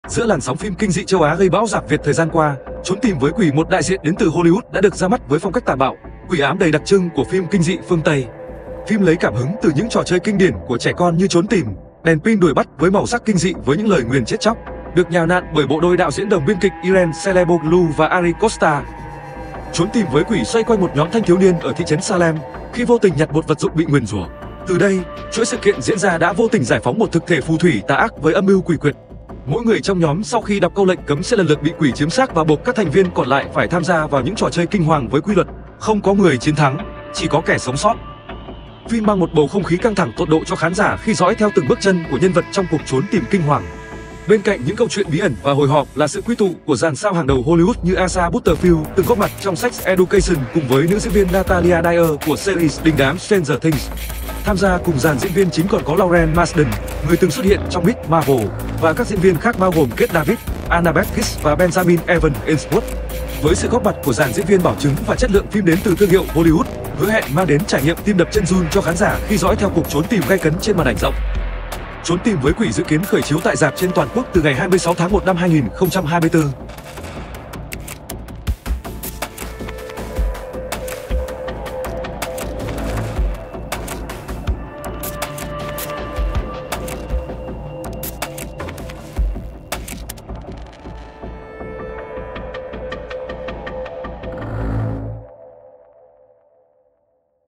Phim kinh dị Trốn Tìm Với Quỷ đại diện Hollywood xông đất phòng vé Việt đầu năm 2024
© Tạp chí Thế Giới Điện Ảnh
☎️ Đặt báo: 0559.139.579 (Nguyên Đạt)